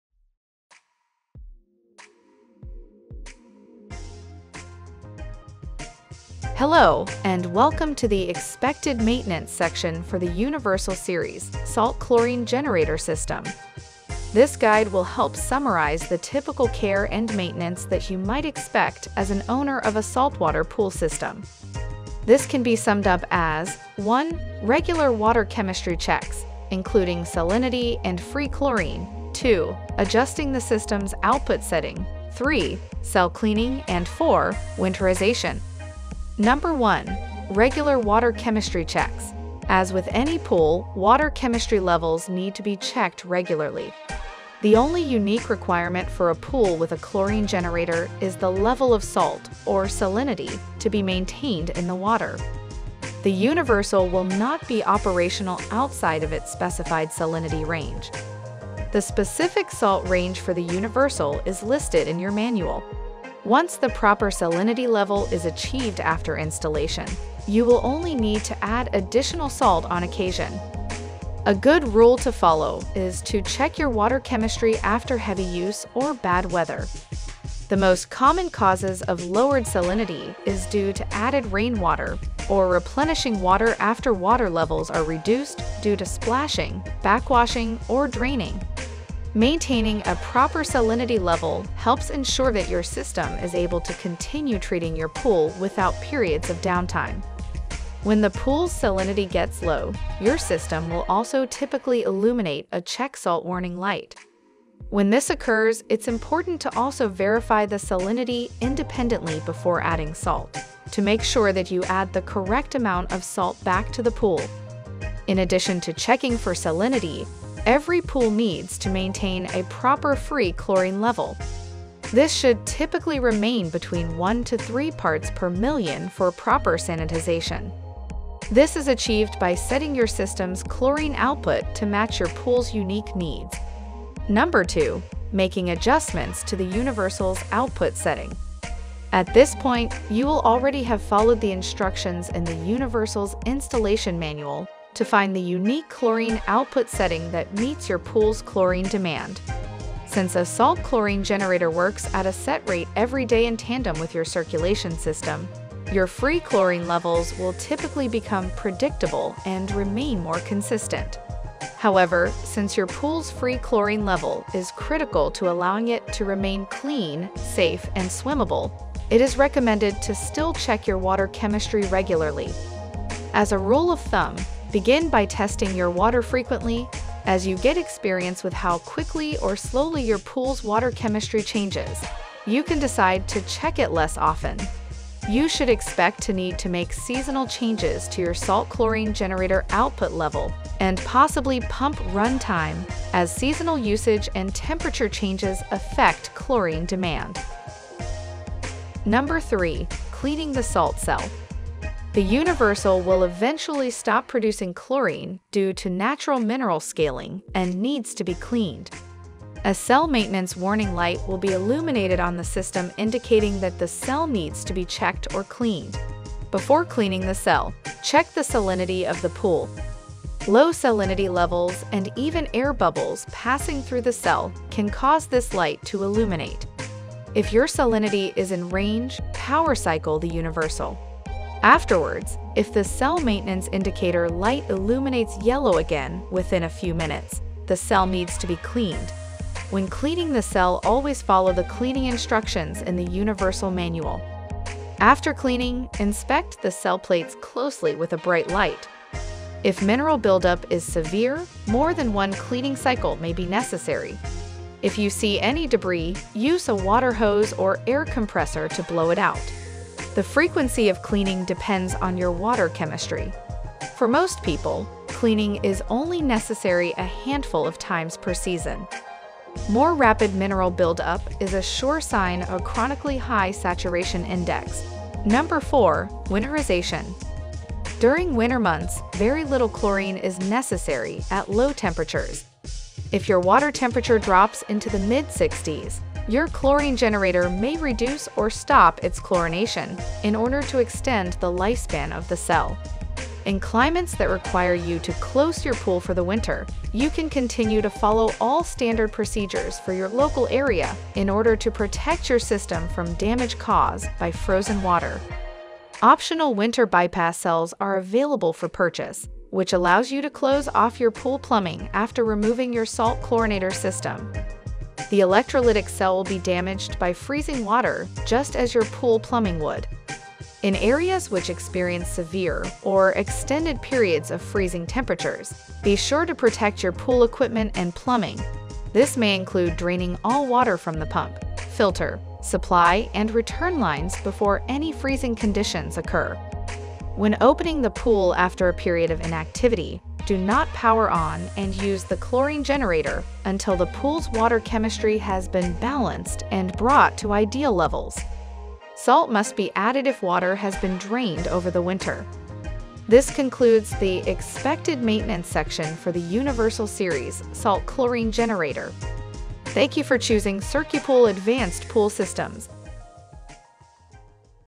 Expected Maintenance Guide | CircuPool Universal Series Salt Chlorine Generator | 4 Basics of Care
This video guide goes over how you should expect to interact with and care for your Universal-Series salt chlorinator system. This guide will prepare you to 1) check your pool water's salinity level and add salt if needed; 2) check your pool water's free chlorine level and adjust your system's chlorine output level as needed, typically seasonally; 3) clean your Universal system's electrolytic cell when natural mineral scaling builds up; 4) protect your Universal system from winter weather and freeze damage.

Please always be sure to have fully read your product's manual for the most complete information and important safety warnings about your product. Questions?

This guide covers the following models (SKU | Model Name):
UL25 | Universal25 Salt Chlorine Generator
UL40 | Universal40 Salt Chlorine Generator
UL55 | Universal55 Salt Chlorine Generator

Questions this guide helps answer:
When do I need to add salt to my pool?
What is the salinity range for the CircuPool Universal system?
How often do I need to test my pool water?
What is the right chlorine output setting for my CircuPool Universal system?
How often do I need to adjust my CircuPool Universal system?
What do I do when the Cell Maintenance warning light is on my CircuPool Universal controls?
How do I know when I need to clean the CircuPool Universal cell?
Does my CircuPool Universal generate chlorine differently when the water is cold?
Do I need to winterize my CircuPool Universal salt pool system?